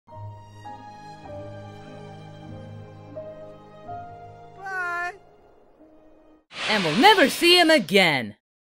What REALLY happened to the Ice Age baby
So much hate towards a character that’s only in one film...